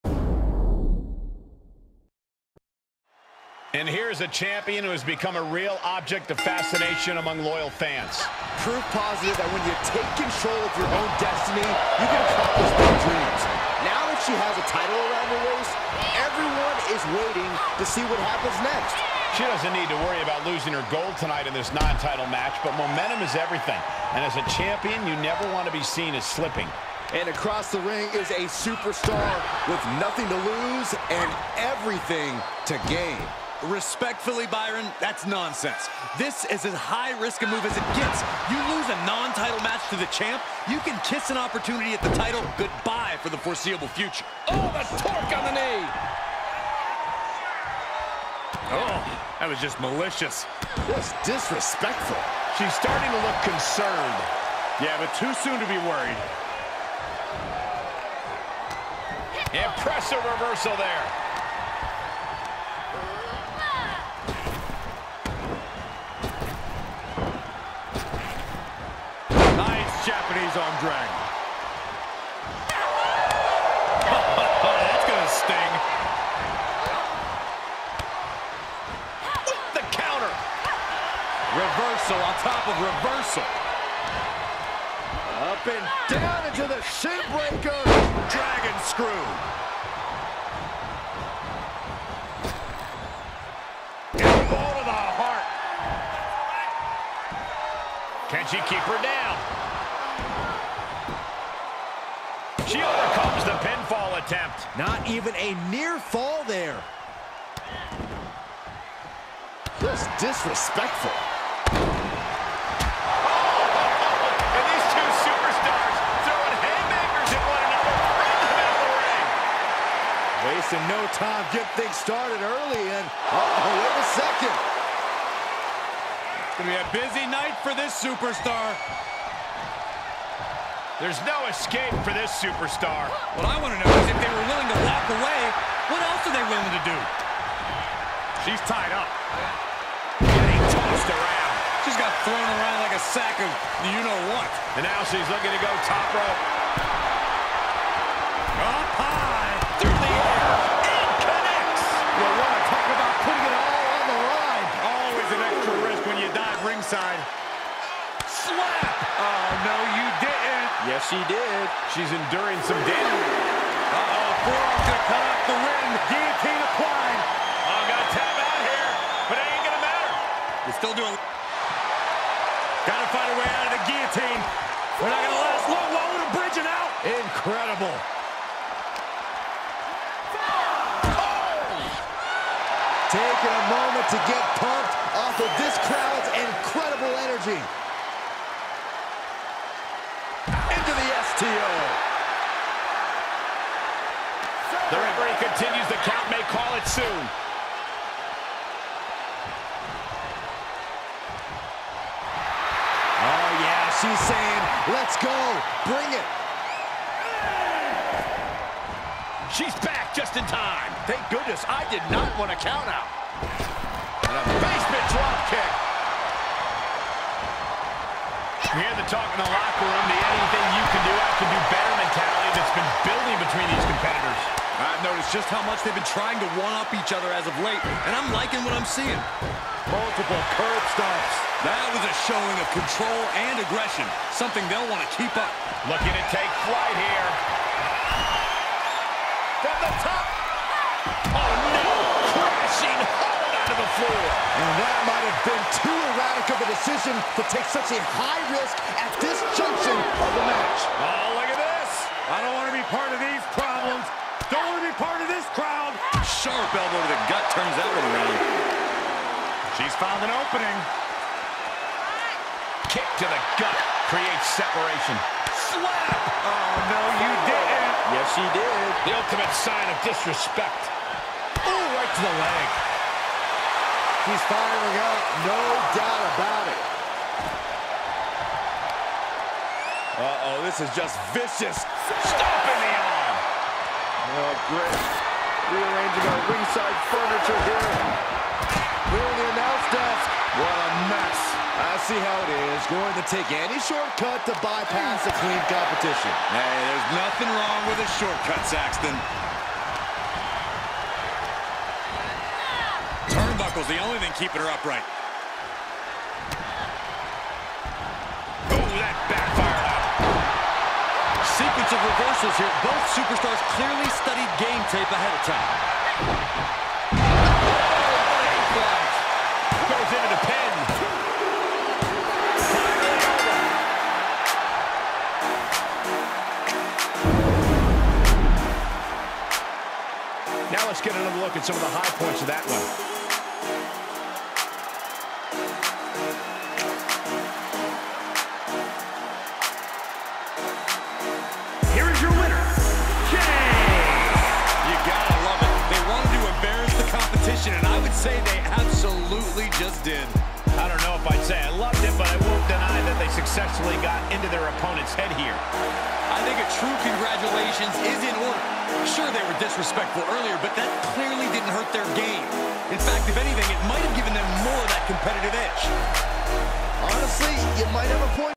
TPJW - Glorious Ep3 - Title Opportunity Match
Jacel VS Urena[c] -  Urena doesn't think Jacel has earned the right to a title match, so Jacel challenges her to a Title Opportunity Match. If she can beat Urena tonight she gest a Title shot at the next PLE, "Day One".